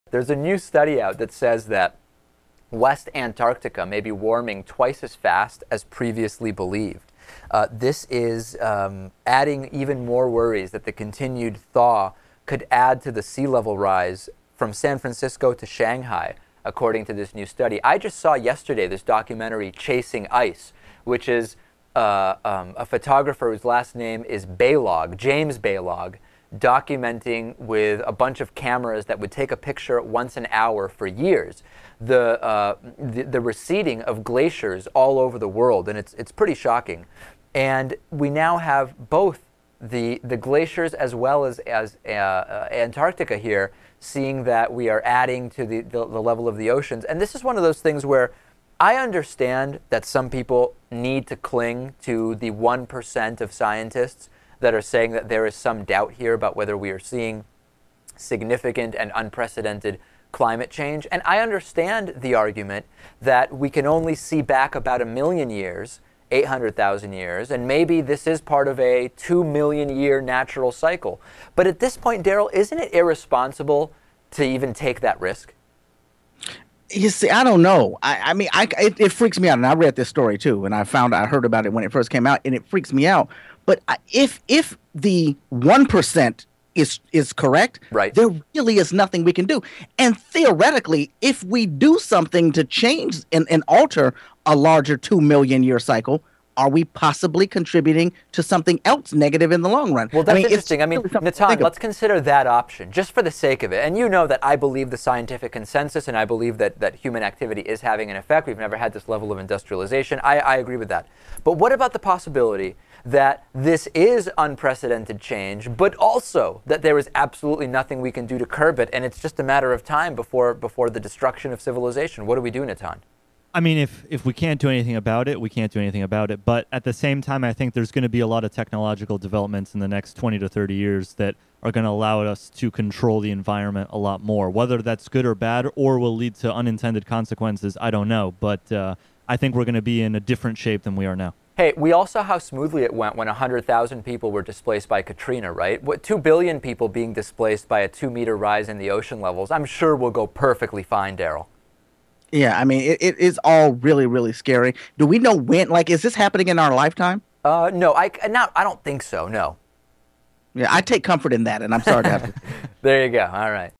West Antarctica Melting Twice As Fast As Originally Thought
--A new study shows west Antarctica is warming twice as fast as previously believed.

--On the Bonus Show: Fake dentist has bathroom office, weirdest world record of 2012, lawsuits over prank calls, more...

24/7 Voicemail Line: (219)-2DAVIDP